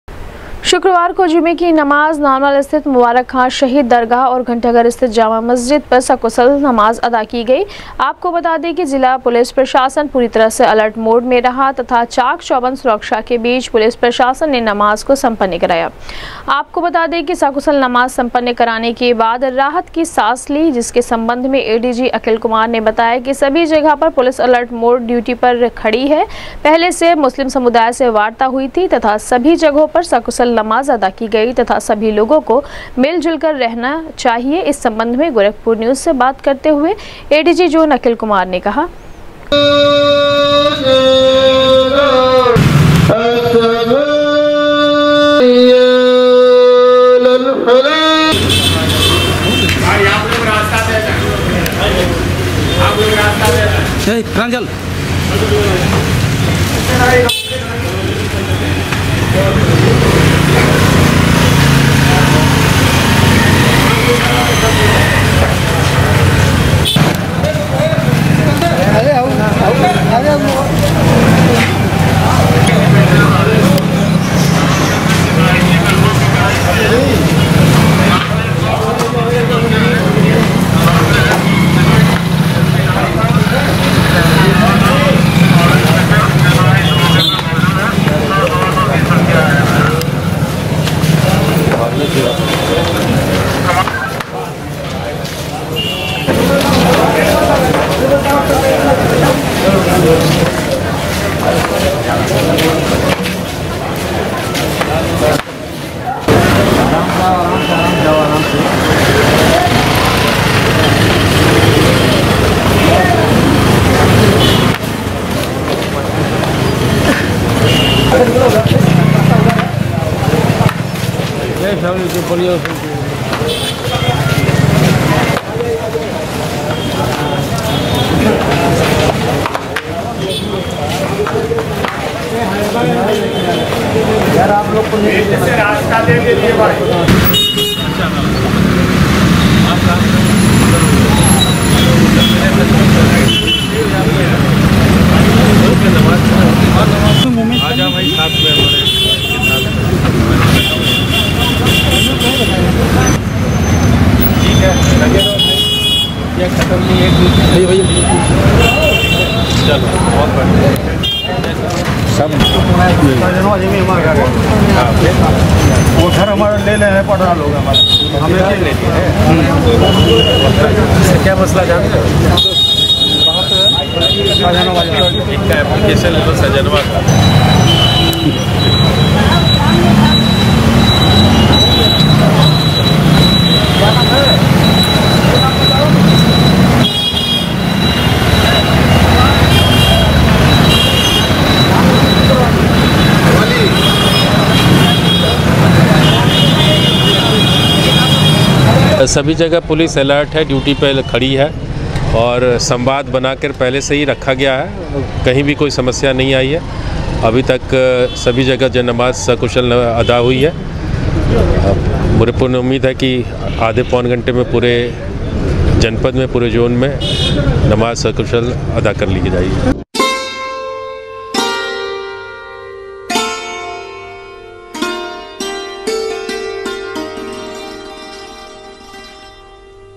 शुक्रवार को चाक चौबंद सुरक्षा के बीच जनपद में सकुशल अदा की गई जुमे की नमाज, पुलिस और प्रशासनिक
शुक्रवार को चाक चौबंद सुरक्षा के बीच जनपद में सकुशल अदा की गई जुमे की नमाज, पुलिस और प्रशासनिक अमले ने ली राहत की सांस, एडीजी जोन अखिल कुमार ने सभी लोगो से मिलजुल कर रहने की,की अपील ।।